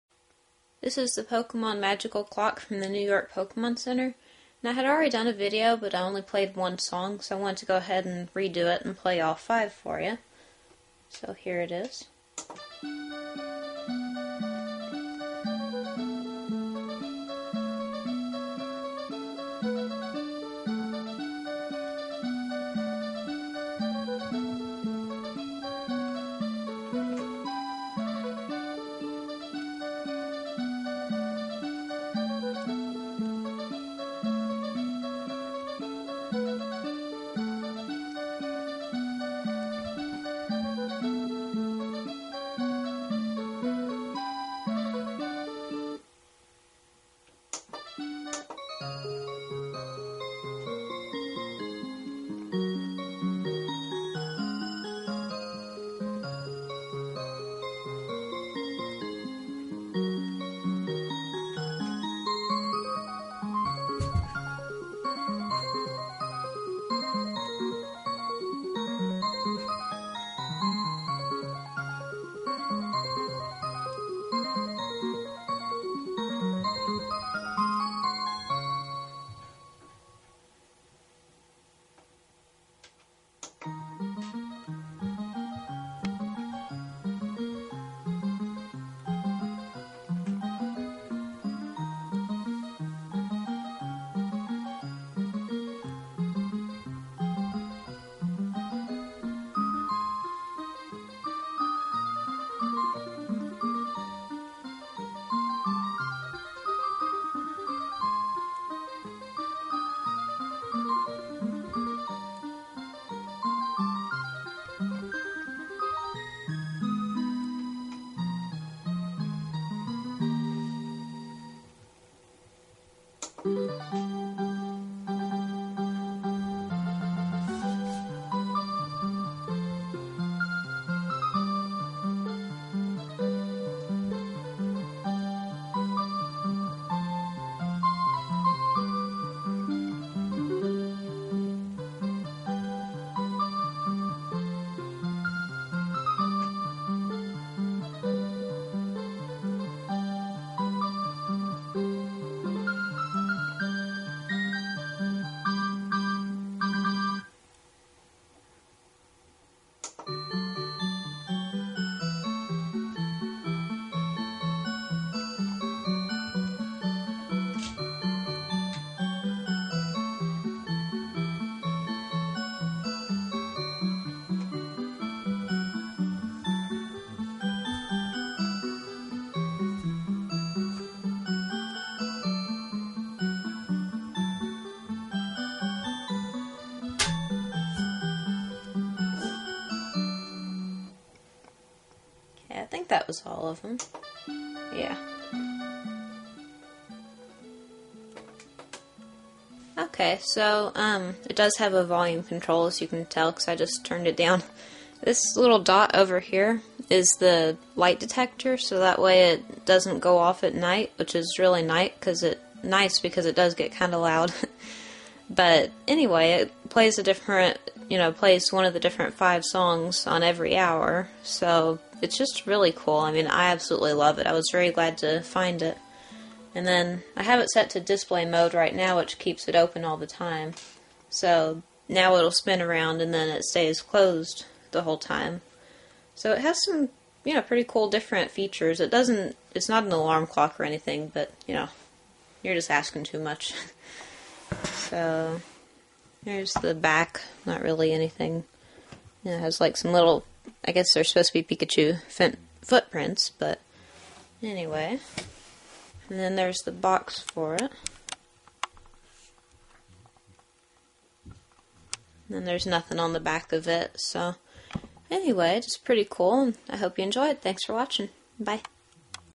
Pokemon Musical Clock - full video with all 5 songs.
Magical musical clock from the New York Pokemon Center. I made a video before and just played one song, so I thought I'd do it again and play all 5 and show the clock in more detail.